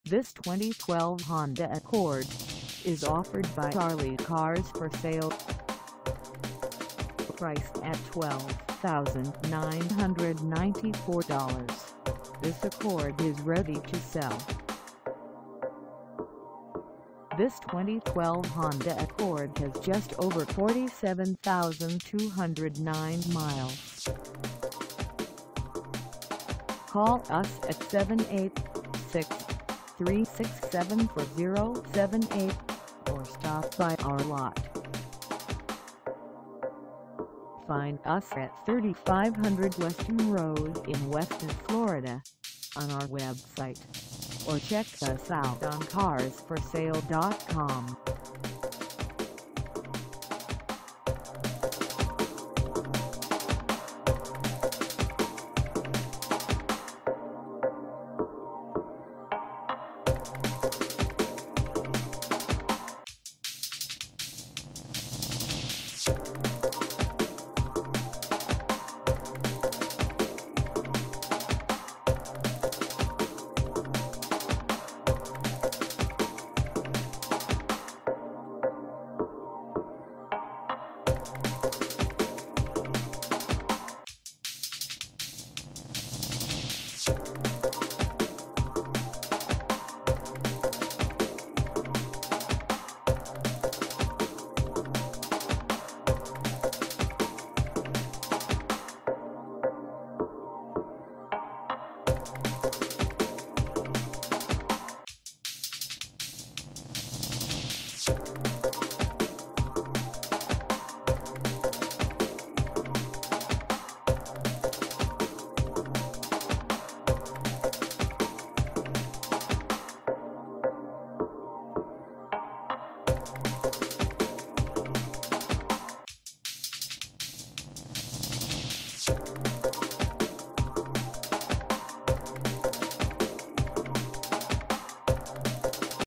2012 Honda Accord for sale in WESTON, FL 33331 at the Ali Ca
This 2012 Honda Accord is for sale in WESTON, FL 33331 at Ali Cars For Sale.
Engine: I4 2.4L I4
Transmission: Automatic 5-Speed
Color: Unspecified
Mileage: 47209
Contact Ali Cars For Sale 3500 Weston Rd WESTON, FL 33331 to test drive this 2012 Honda Accord  today.